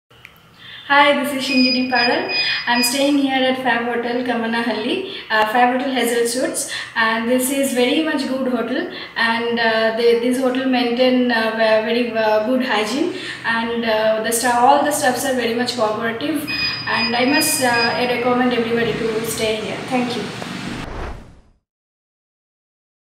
Ms Shinjini Paral @ FabHotel Hazel Suites, Bangalore | Guest Testimonial
Ms. Shinjini Paral shares her experience at FabHotels!